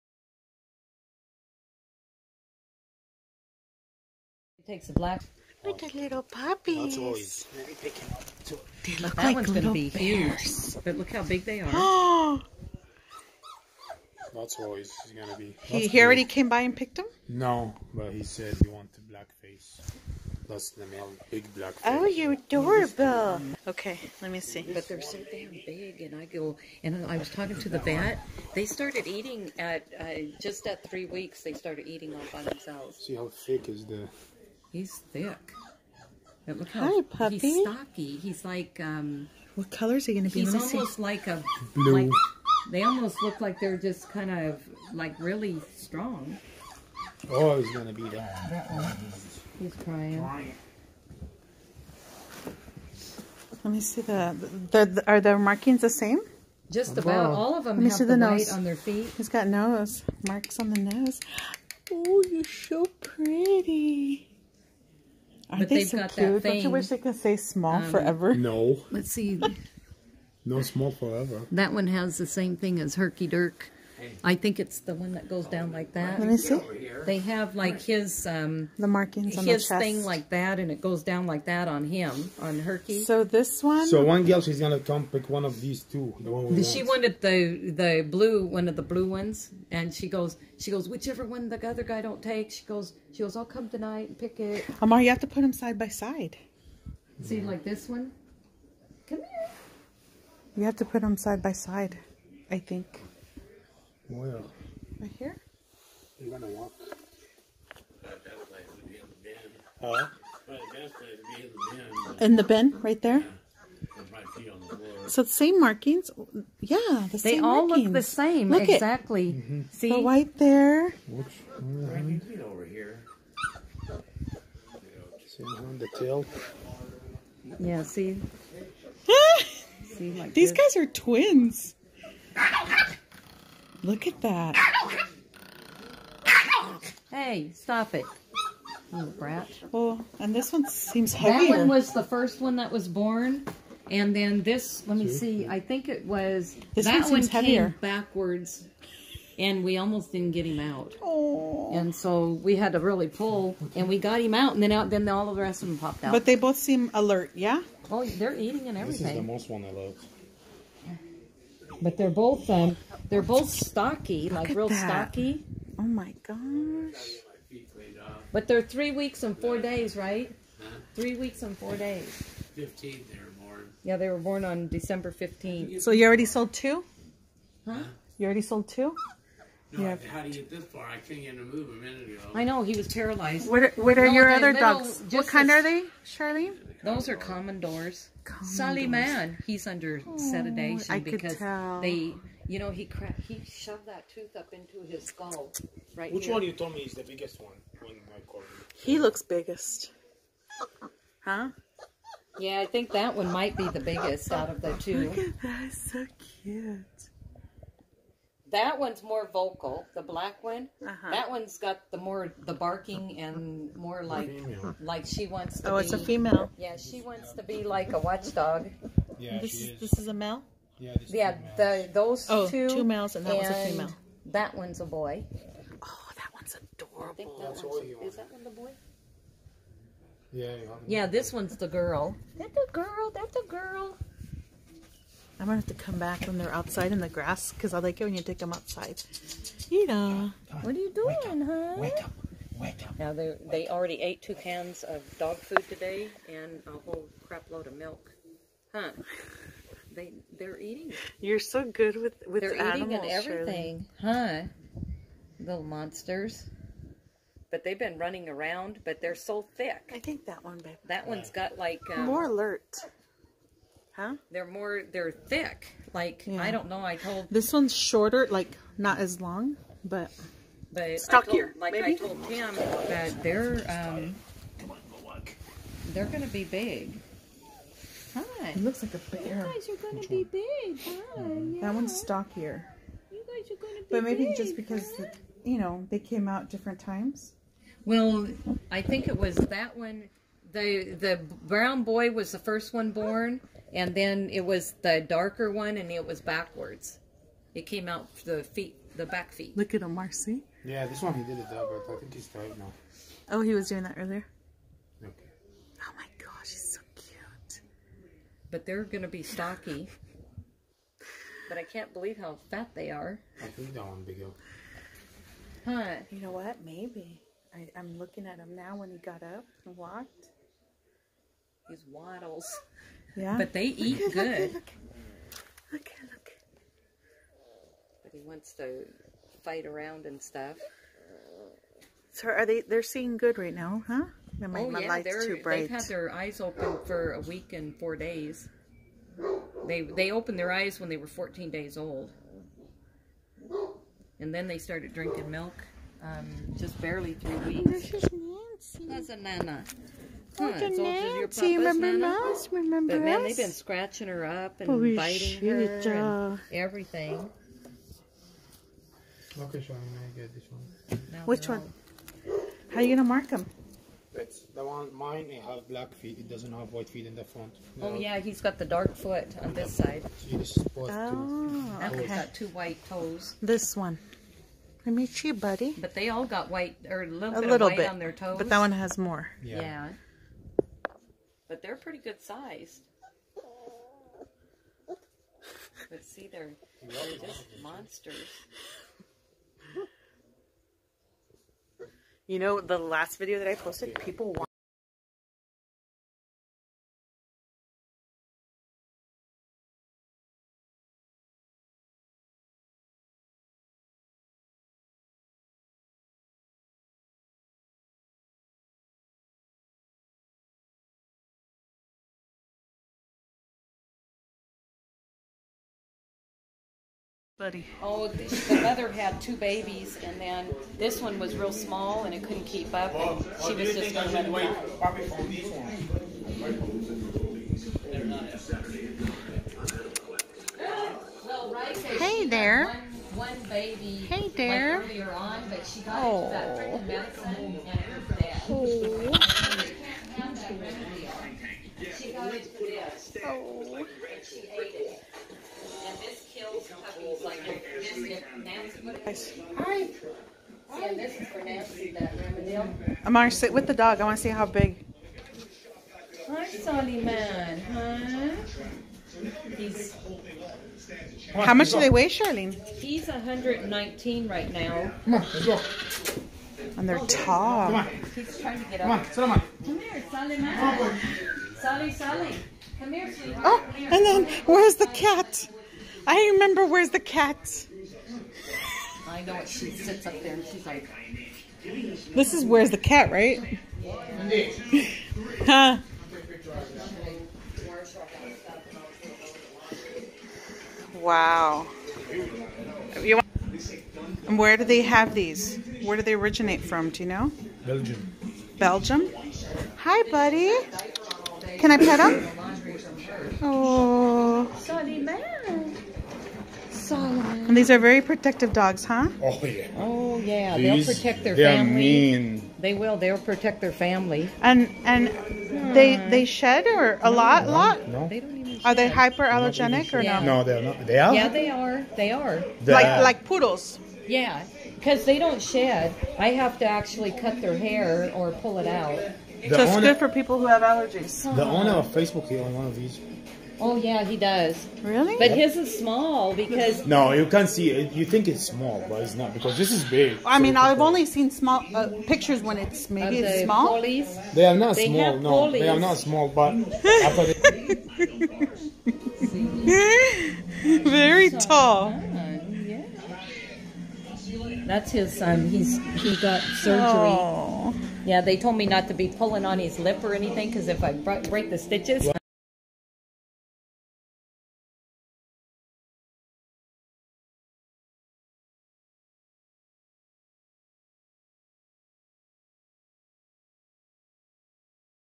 Cute Puppies French Mastiff, But see Komondor Dogs!
Cute Puppies French Mastiff are 3 weeks! About 4 are left to buy in Yakima Washington.  Message me if you're interested to buy or ask questions!   In this video you will also Komondor Dogs that she plans to breed as well.